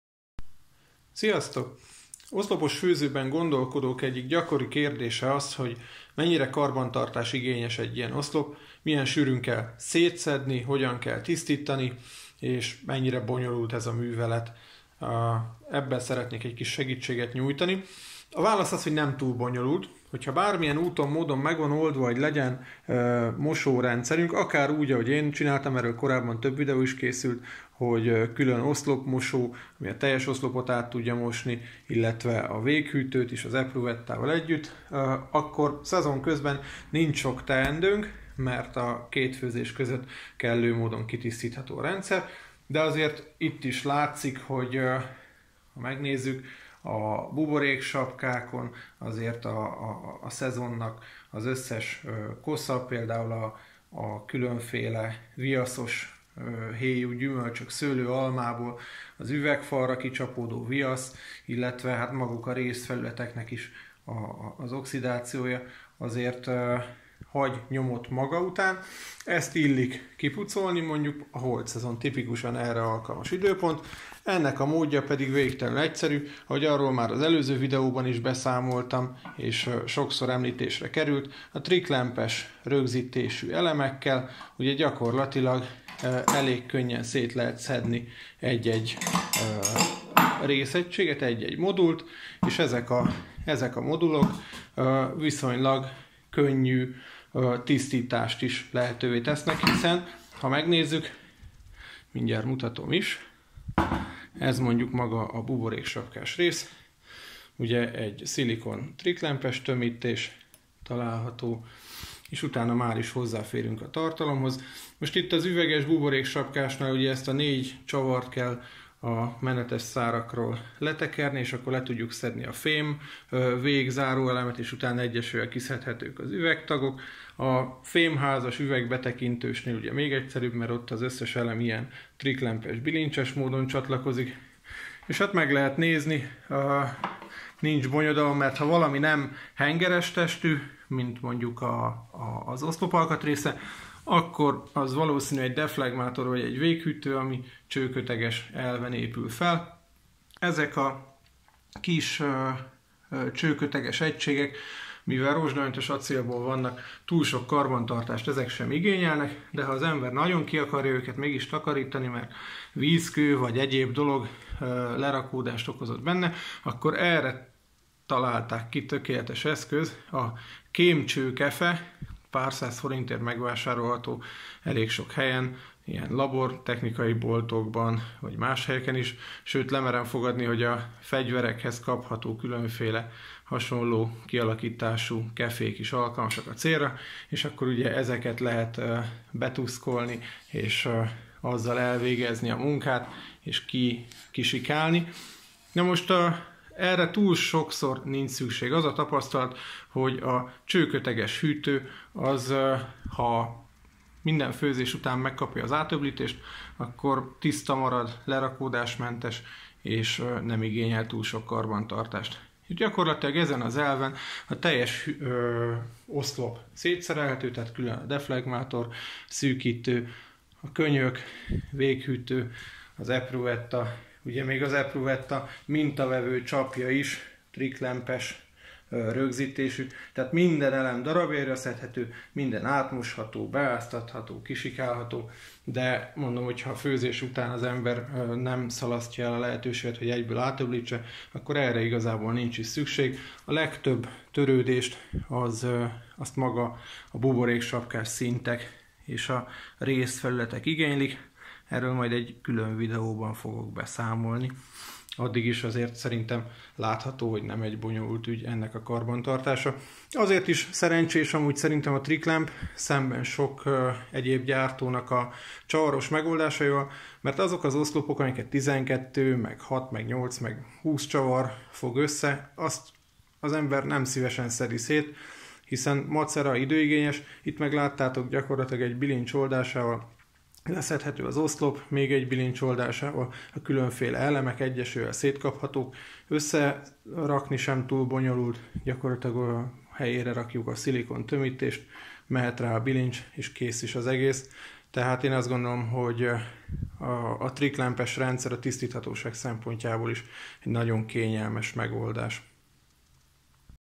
Egylépcsős oszlopos pálinkafőző 28. - szétszerelés, tisztítás alapkérdései
Oszlopos főzővel kacérkodók gyakran kérdezik tőlem, hogy milyen sűrűn szoktam szétszedni az oszlopot, és mennyire bonyolult művelet mindez. A mostani videóban erre adok választ és tippeket.